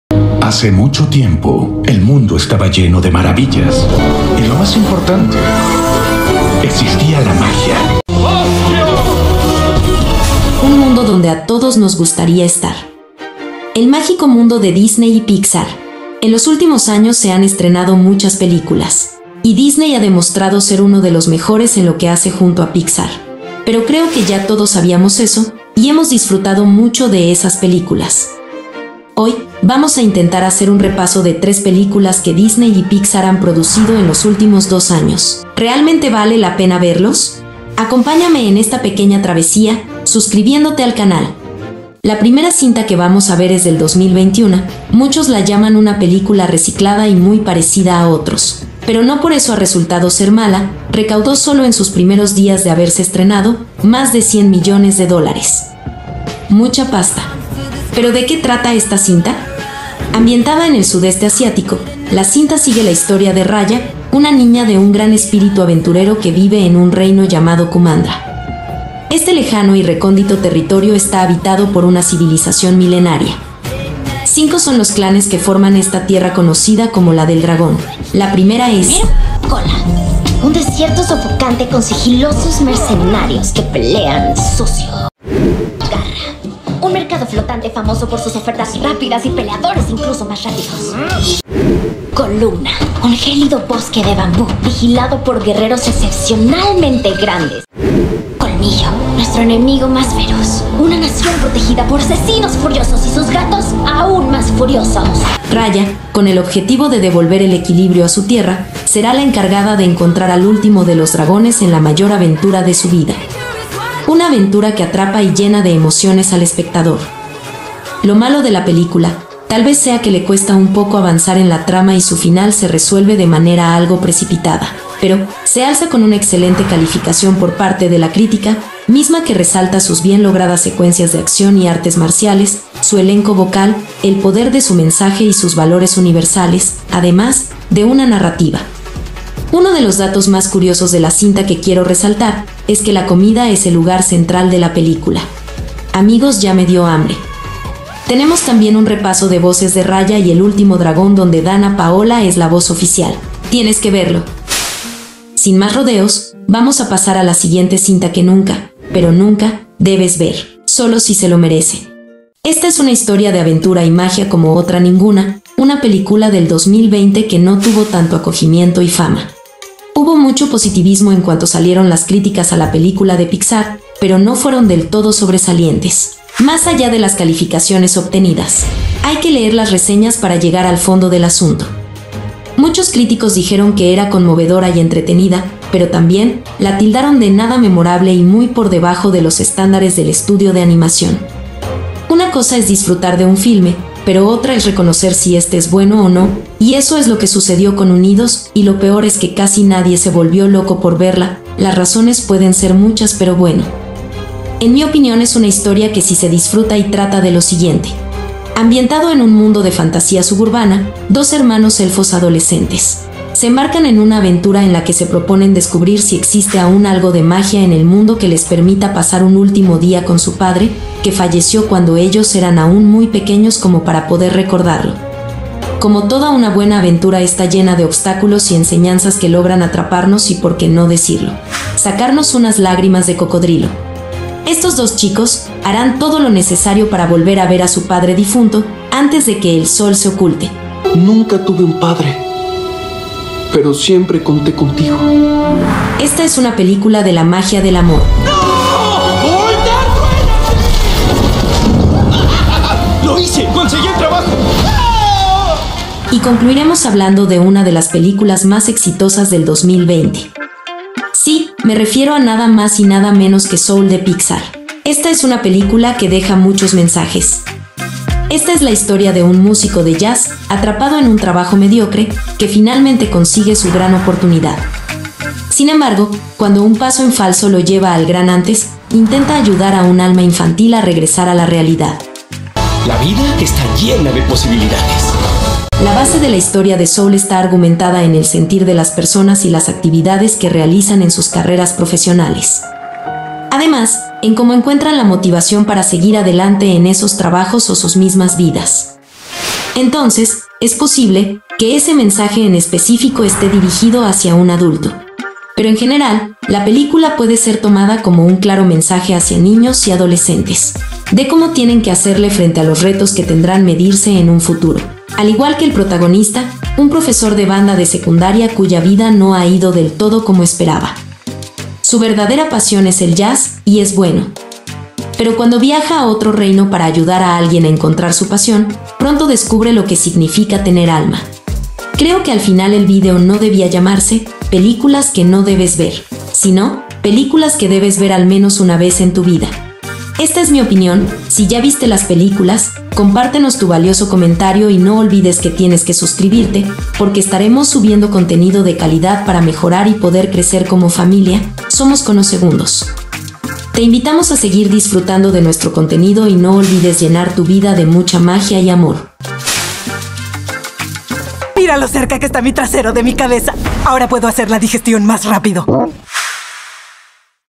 ⚡TOP 3 películas de DISNEY plus 💥 ¿valen la pena?❌
FAMILIA de conosegundos💚, hoy vamos a hacer un repaso de  3 películas de disney y pixar ¿vale la pena verlos?